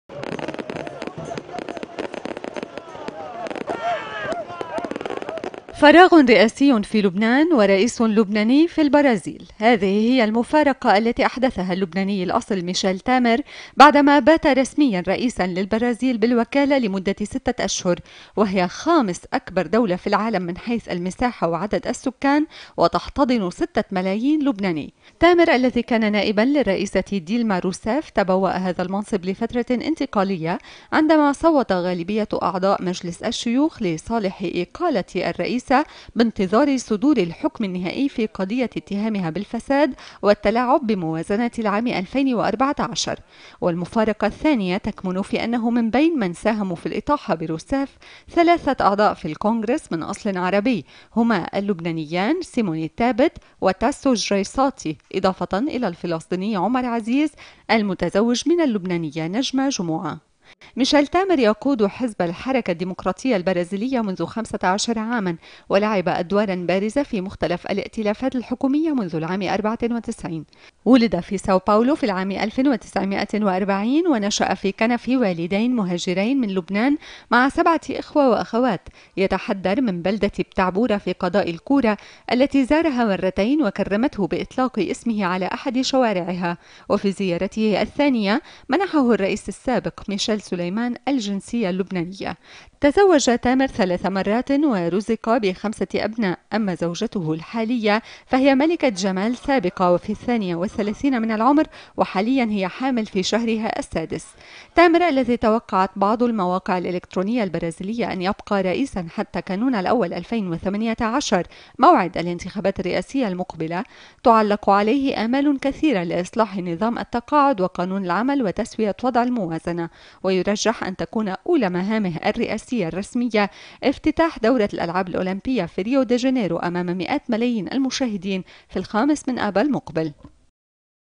فراغ رئاسي في لبنان.. ورئيس لبناني في البرازيل
فراغ رئاسي في لبنان.. ورئيس لبناني في البرازيل.. هذه هي المفارقة التي احدثها اللبناني الاصل ميشال تامر بعدما بات رسميا رئيسا للبرازيل بالوكالة لمدة ستة اشهر، وهي خامس اكبر دولة في العالم من حيث المساحة وعدد السكان وتحتضن ستة ملايين لبناني.

تامر الذي كان نائبا للرئيسة ديلما روسيف، تبوأ هذا المنصب لفترة انتقالية، عندما صوت غالبية اعضاء مجلس الشيوخ لصالح تعليق ولاية الرئيسة بانتظار صدور الحكم النهائي في قضية اتهامها بالفساد والتلاعب بموازنة العام الفين واربعة عشر.

والمفارقة الثانية تكمن في انه من بين من ساهموا في الاطاحة بروسيف، ثلاثة اعضاء في الكونغرس من اصل عربي هما اللبنانيان سيموني تابت وتاسّو جريصاتي، إضافة إلى الفلسطيني عمر عزيز المتزوج من اللبنانية نجمة جمعة.

ميشال تامر يقود حزب الحركة الديموقراطية البرازيلية منذ خمسة عشر عاما، ولعب ادوارا بارزة في مختلف الائتلافات الحكومية منذ العام اربعة وتسعين. ولد في ساو باولو في العام الف وتسعمئة واربعين  نشأ في كنف والدين مهاجرين من لبنان مع سبعة اخوة واخوات، يتحدر من بلدة بتعبورة في قضاء الكورة التي زارها مرتين وكرمته باطلاق اسمه على احد شوارعها. وفي زيارته الثانية منحه الرئيس السابق ميشال سليمان الجنسية اللبنانية.

تزوج تامر ثلاث مرات ورزق بخمسة ابناء، اما زوجته الحالية فهي ملكة جمال سابقة في الثانية والثلاثين من العمر، وحاليا هي حامل في شهرها السادس.

تامر الذي توقعت بعض المواقع الالكترونية البرازيلية ان يبقى رئيساً حتى كانون الاول الفين وثمانية عشر موعد الانتخابات الرئاسية المقبلة، تعلق عليه آمال كثيرة لاصلاح نظام التقاعد وقانون العمل وتسوية وضع الموازنة. ويرجح ان تكون اولى مهامه الرئاسية الرسمية افتتاح دورة الالعاب الاولمبية في ريو دي جانيرو امام مئات ملايين المشاهدين في الخامس من آب المقبل.